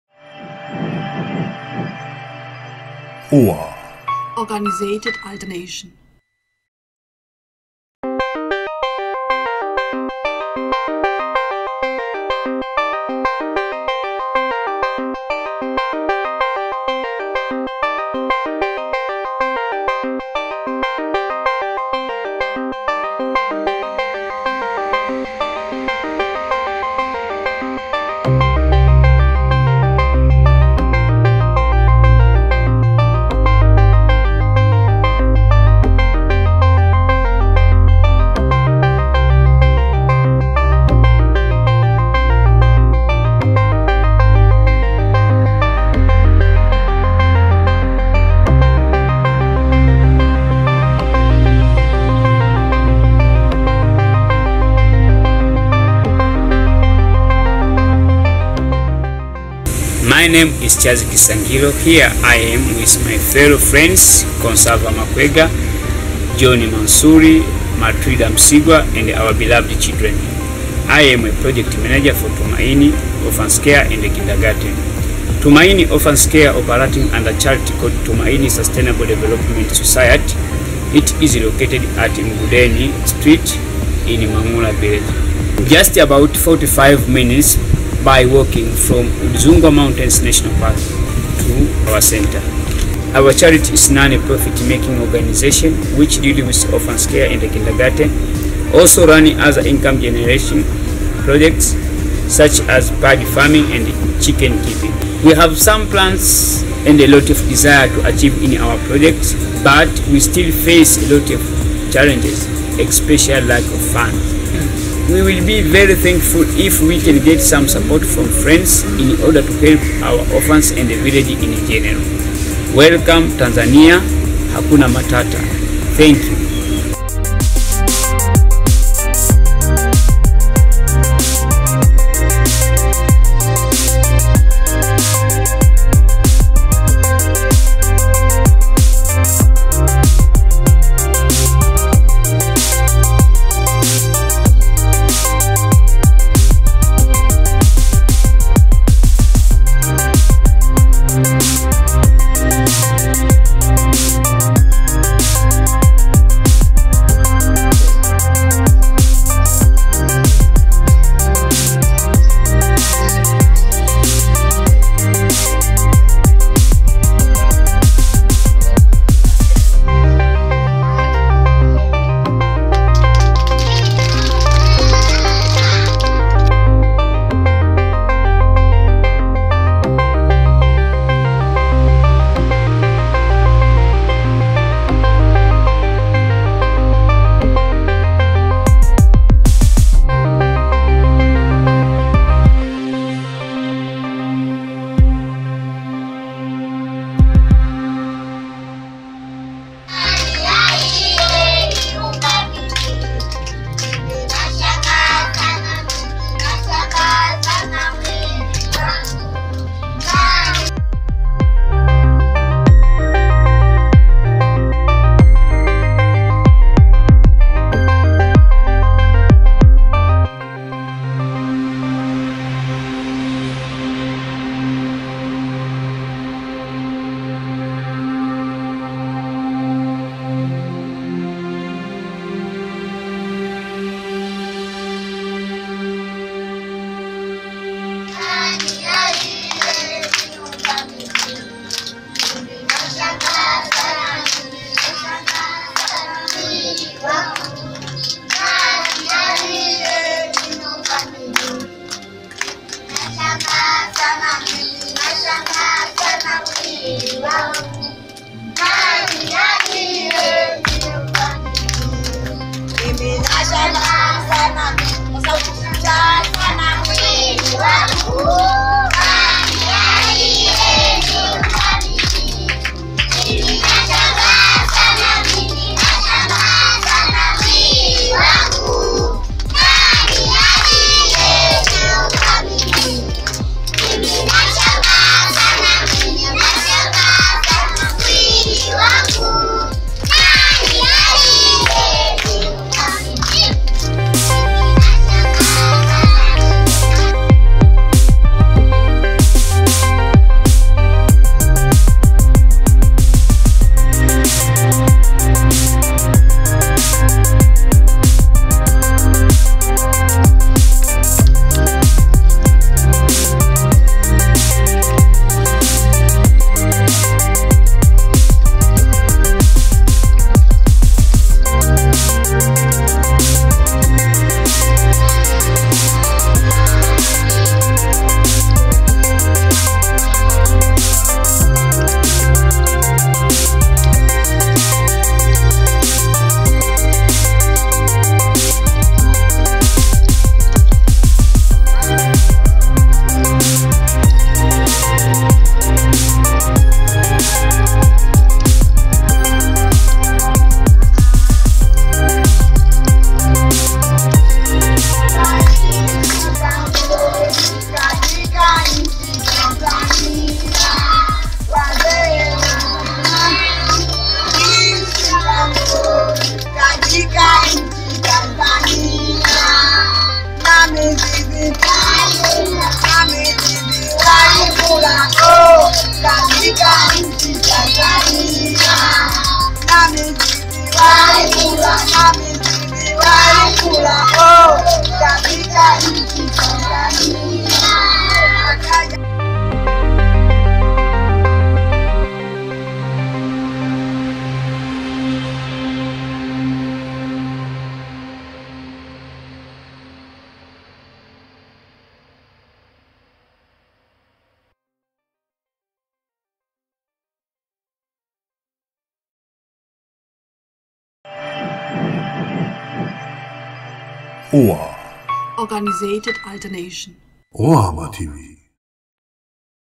charles kisangiro´s orphankids project
this is charles kisangiro from tanzania. he have a group from over 60 orphankids in the age from 3 to 6 years. please support him with all your power.
here you can contact him.